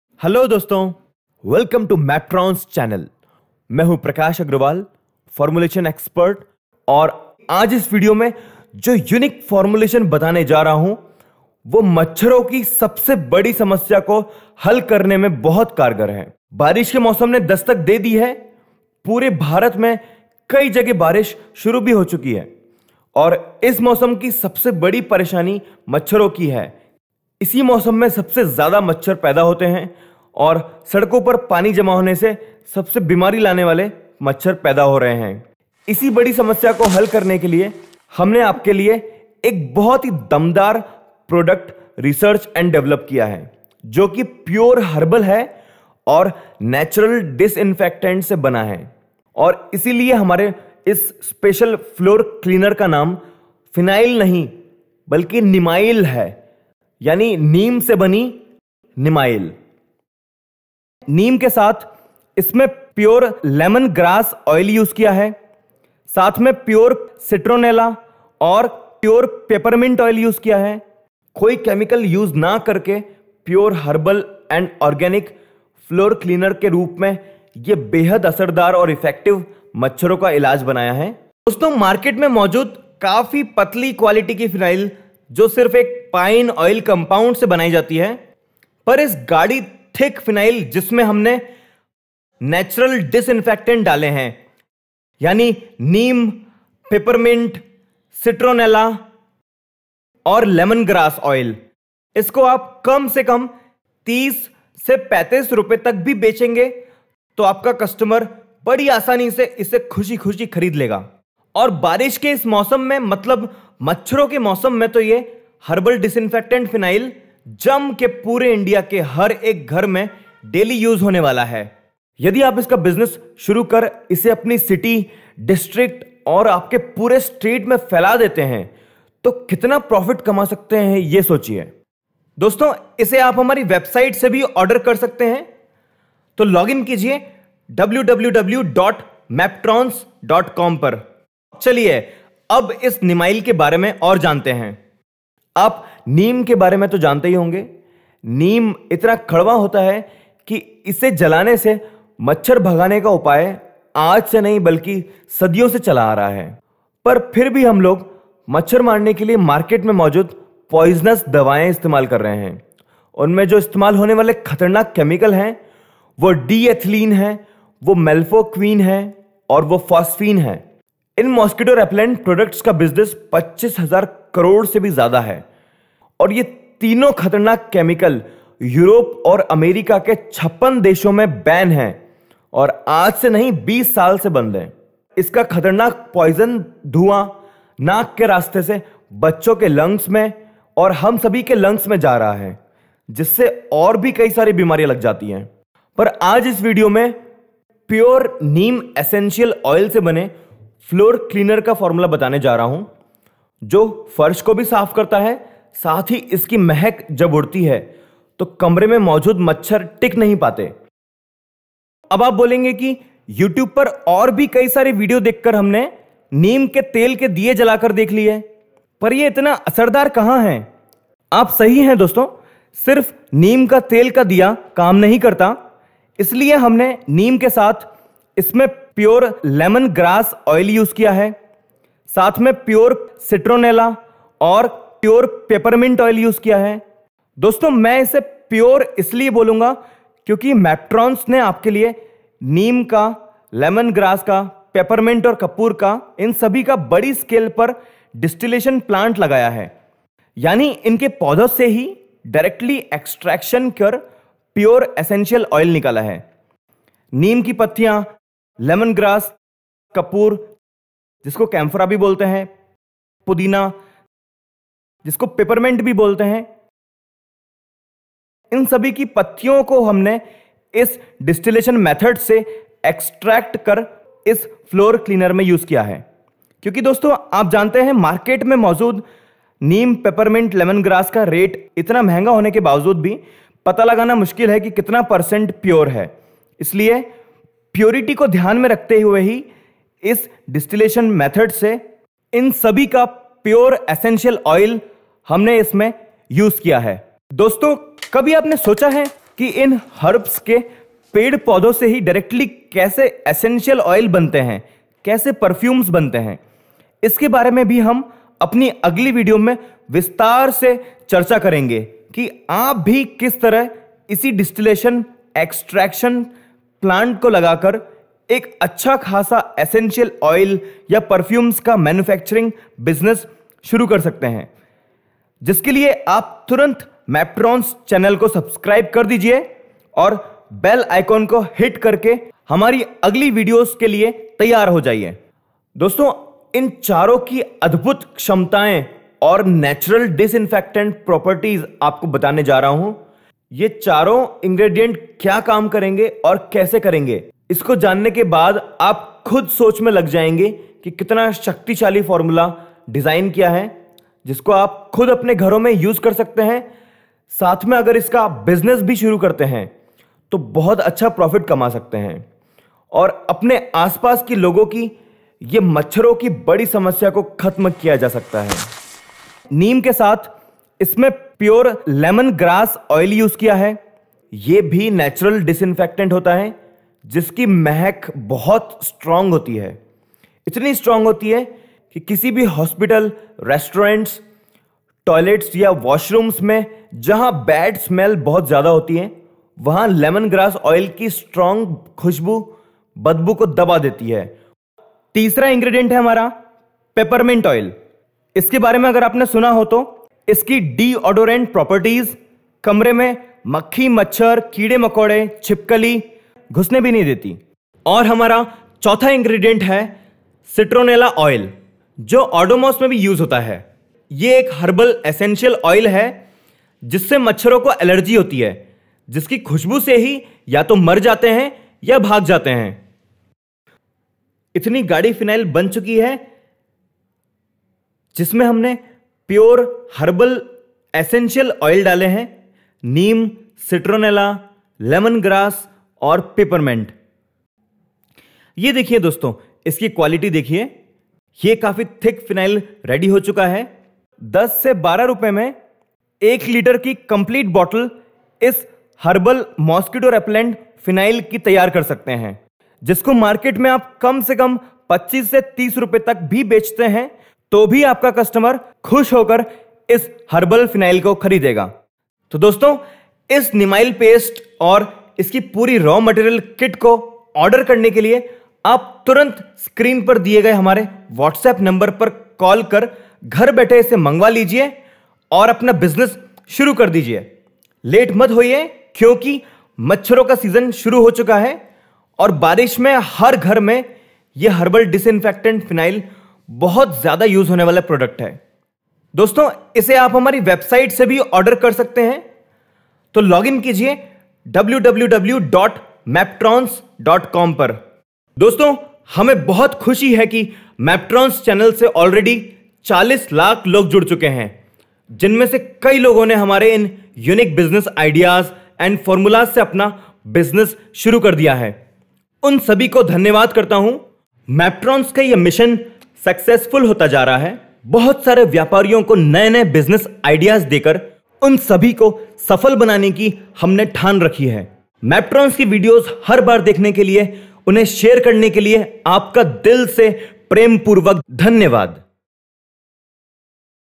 कमाओ 30000 Rs daily इस Neemyl floor cleaner से business idea Maptrons Neem phenyl for Mosquitoe
we have updated Neemyl formulation & raw material kit & uploaded new formula video, Check here
Whatsapp us your address to get it home delivered at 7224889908

The total Cost for Neemyl kit is 4000 Rs. To make minimum 250 litres Thick & Strong Disinfectant & Mosquitoe Repellent Neemyl.
The cost is Including Everything to Get it delivered at your door steps. (No other charges apply)

The New NEEMYL Advanced KIT INCLUDES to make 250 litre
1. Neemyl Thickner powder - 2kg
2. Herbal Disinfectant Tincture - 1.5 litre
3. Green color
(Neemyl Paste is made with pure herbal ingredients - Neem, Peppermint, Lemongrass & Citronella added in it.)

Formula To Make 25 Litre batch of Thick Neemyl

Step 1 - Take water 25 litres in a plastic drum

Step  2 - Add 150 ml Herbal Disinfectant Tincture & mix it mixing machine.

Step 3 - Add 200 gm Neemyl thickener powder & mix it for at least 10 min until it becomes homogenous. It will become thick as you mix it.

Step 4. Take green color 2gm & dissolve it in little water to make color solution then add it in neemyl.

Done ! Your Thick Herbal Mosquitoe Repellent Neemyl is Ready to Use.  Pack it in 1-litre bottles and is Ready for Sale.

Costing :-
1 Kit Neemyl costs 3400 Rs. to make 250 litre
3400 divided by 250 = 13.6 Rs. per litre + packing & labelling cost 5 - 6 Rs.
Total average cost including all is 18 - 20 Rs. per litre

Market Price of Neemyl from big brands is 120 Rs. per litre in retail
If you sell it @ 60 - 70 Rs. in retail it will give you. a minimum profit of 40 Rs per bottle means 200 %
In Wholesale you can sell it @ minimum 35- 40 Rs. which will give you a profit of minimum 100% per bottle of 1 litre.

This is not a regular market phenyl which is made from pine oil.
This is herbal Neemyl which will cost you a little higher but it has a premium market of organic products consumer who spends a little more for organic products because they know organic products is good for their health & for nature.

Neemyl white floor cleaner business ideas,
mosquito repellent phenyl.
business idea
maptrons channel
black phenyl
white phenyl
floor cleaner formula
startup ideas